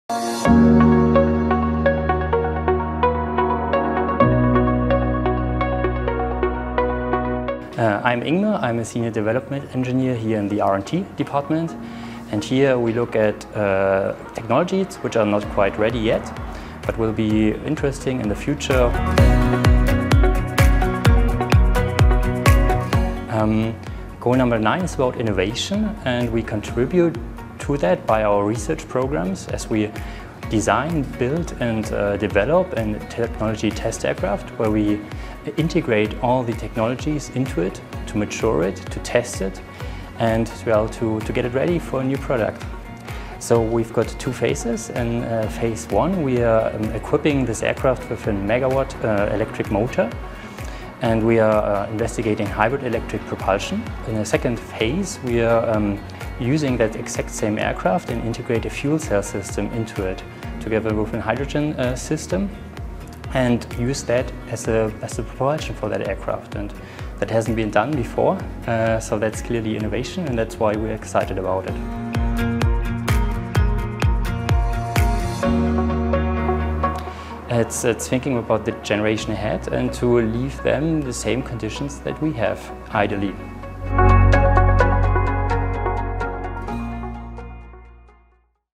SDG: Ep. 3 | Ingmar Geiß, Senior Development Engineer | Industry, Innovation & Infrastructure
We have reached the end of the week focusing on how Deutsche Aircraft aligns its work on some of the SustainableDevelopmentGoals. Ingmar Geiß, our Senior Development Engineer, explains how Deutsche Aircraft is using innovative technologies to achieve our goals of a more sustainableaviation in our RaceToZero.